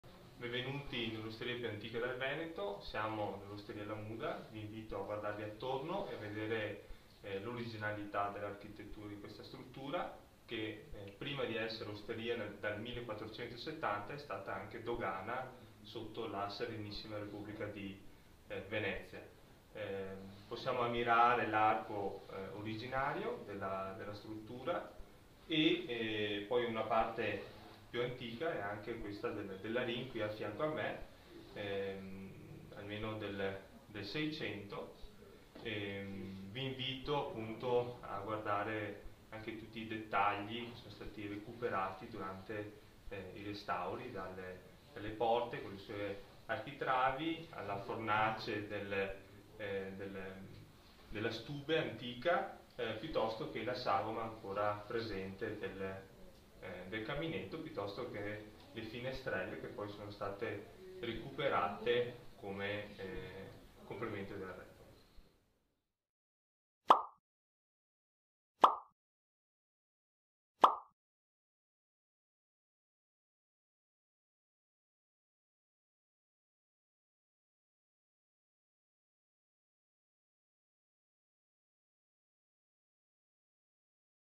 Osteria La Muda - La più antica osteria del Veneto, dal 1470 d.C. - 360 3D
Antica Osteria La Muda, passo San Boldo (TV), dal 1470 d.C.
Selezionare la risoluzione 4K.

Grazie a Osteria La Muda.